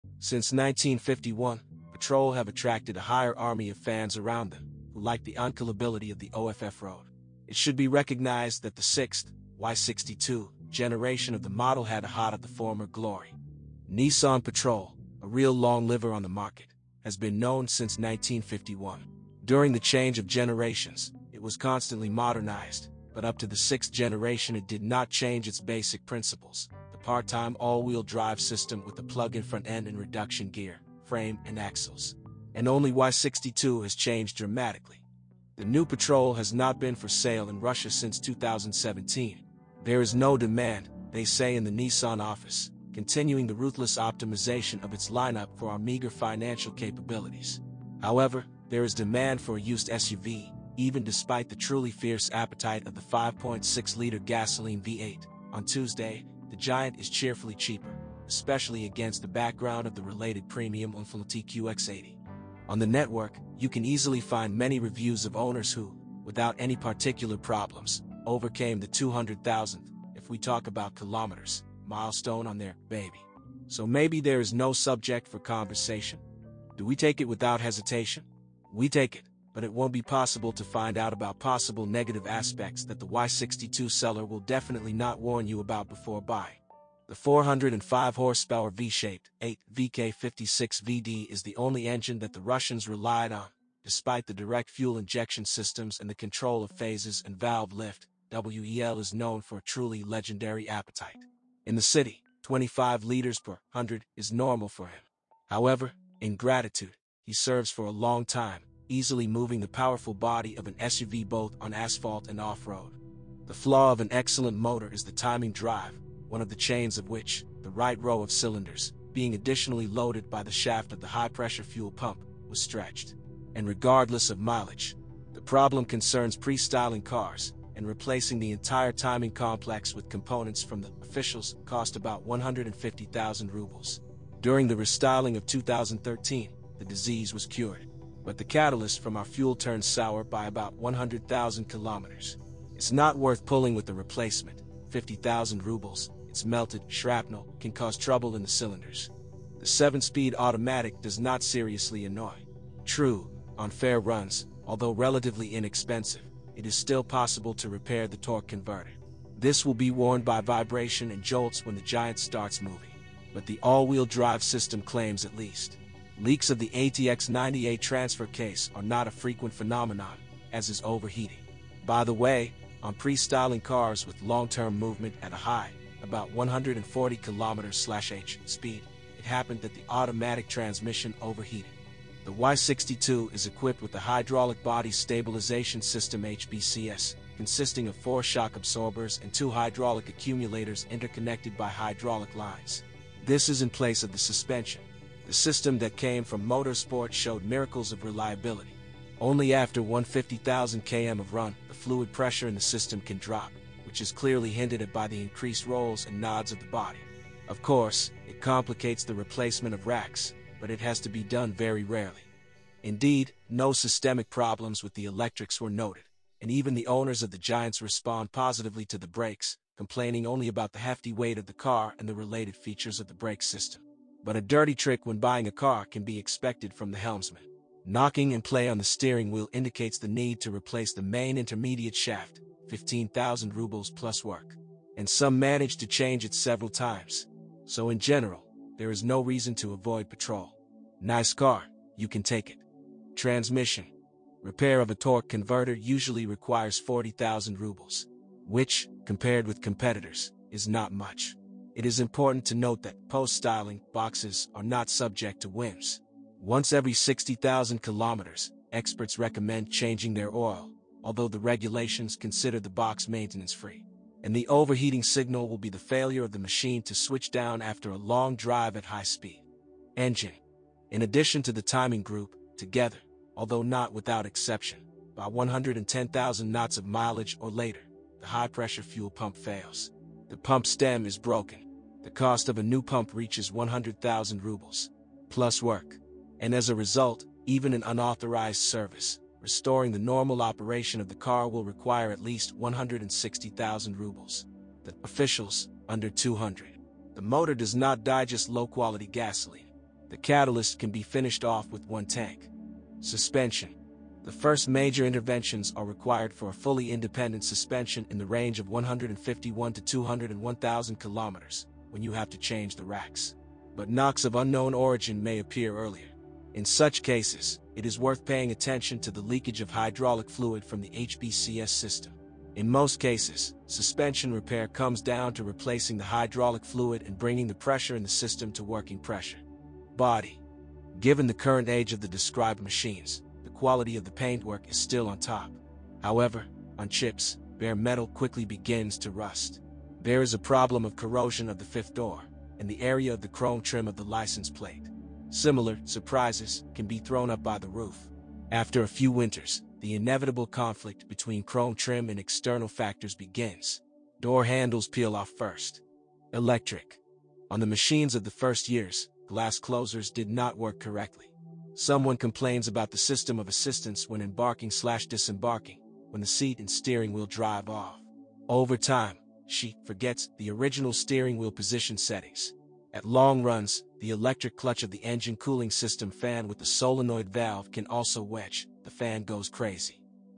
What are the cons of owning a Nissan Patrol 6 generation?
The most common problems with a used Nissan Patrol VI generation?